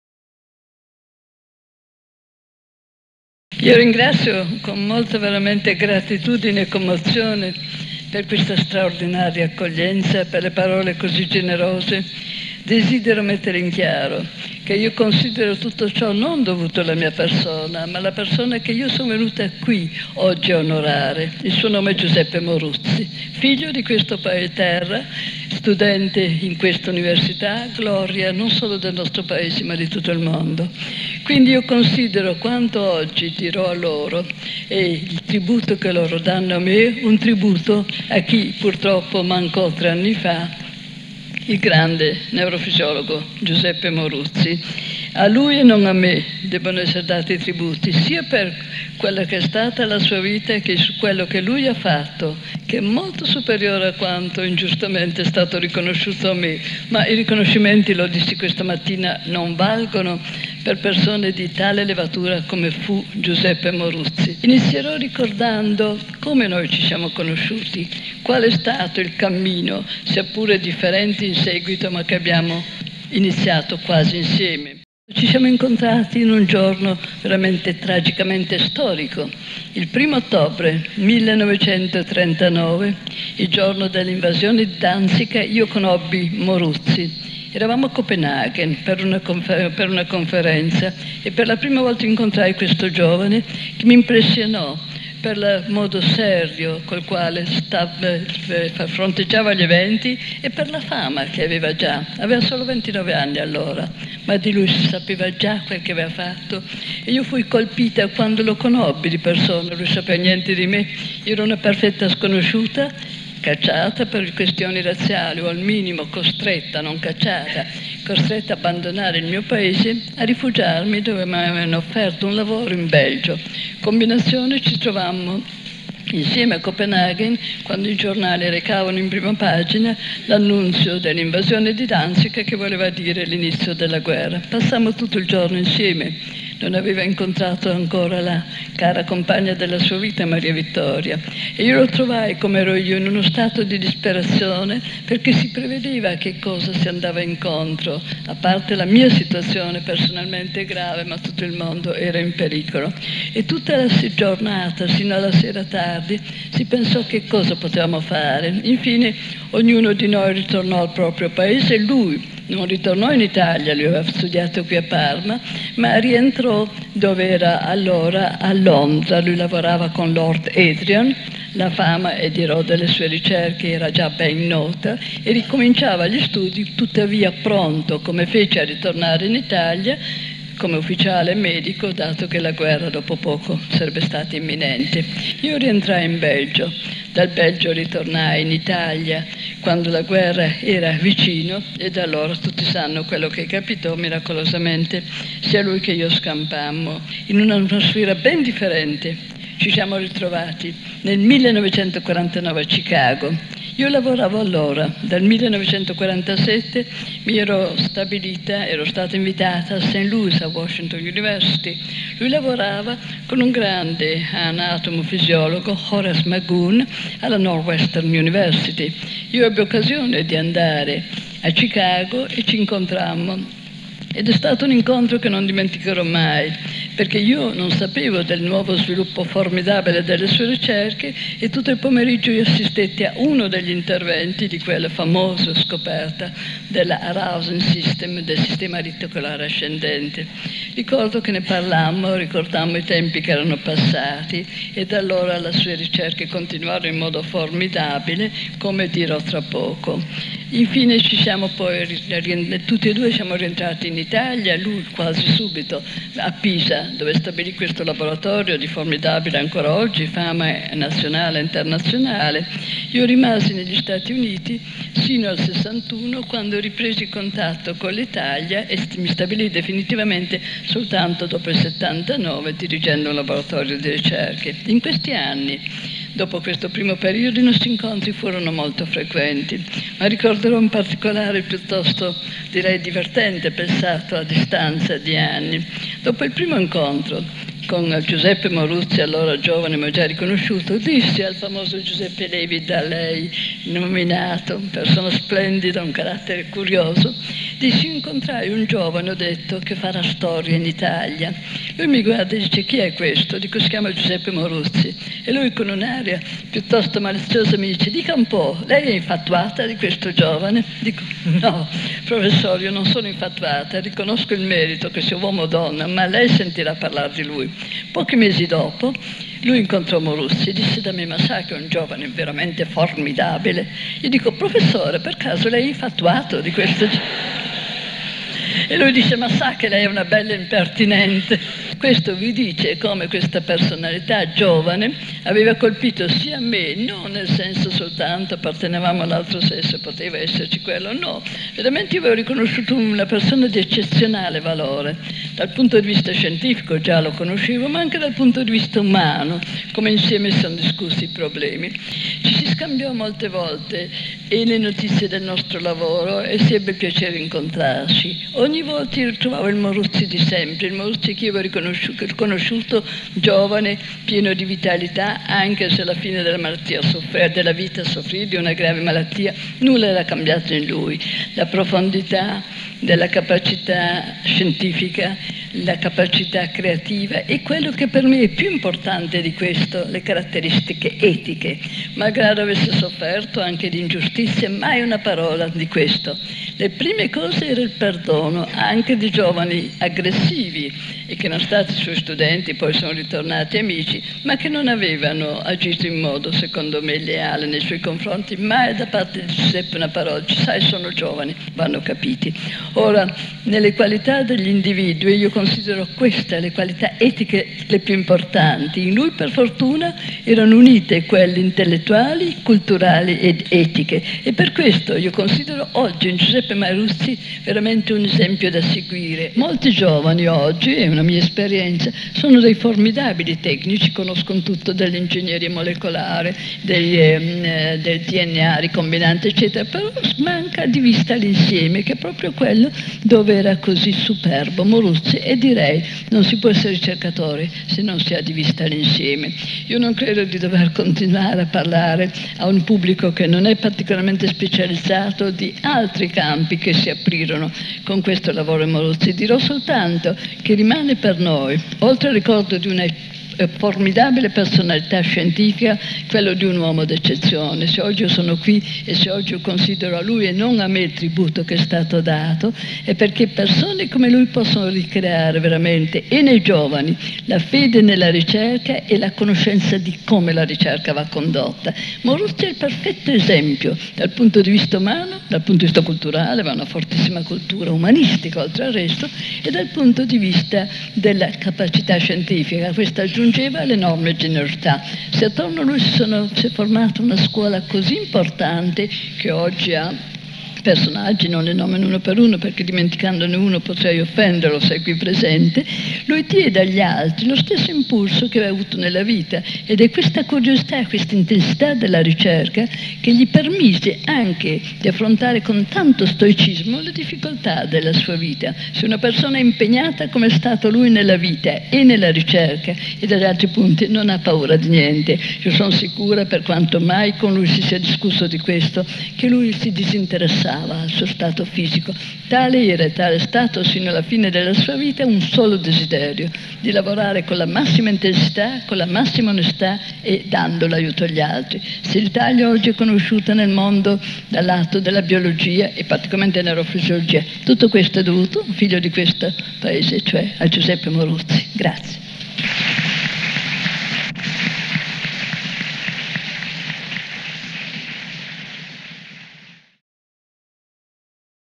Rita Levi Montalcini ricorda Giuseppe Moruzzi - English subtitles
Rita Levi Montalcini remembers Giuseppe Moruzzi,  University of Parma, 20 marzo 1990.

Questo intervento è integralmente riprodotto nel libro "Giuseppe Moruzzi. Ritratti di uno scienziato - Portraits of a scientist", Edizioni ETS. (Ita-Eng text)

Con scritti di [with writings of]: Rita Levi-Montalcini, Mario Tobino, Michel Meulders, Paolo Moruzzi, Marco Piccolino, Giovanni Bottaro, Brunello Ghelarducci, Pier Francesco Listri.